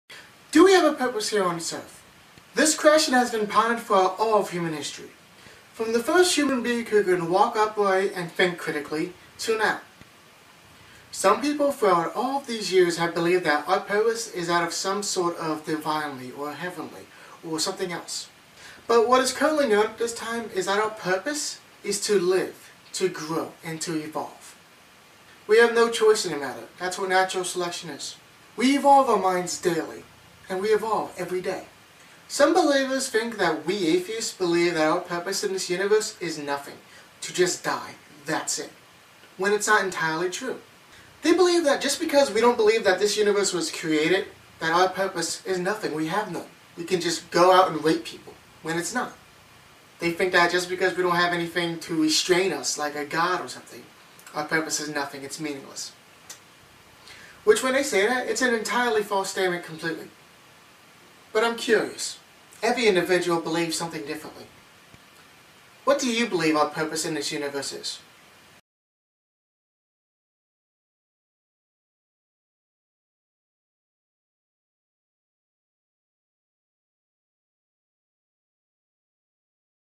Purpose
Do we have a purpose? Believers think its from a divine being that created us/ our purpose, then they also claim we atheist have no purpose because we dont believe in a god...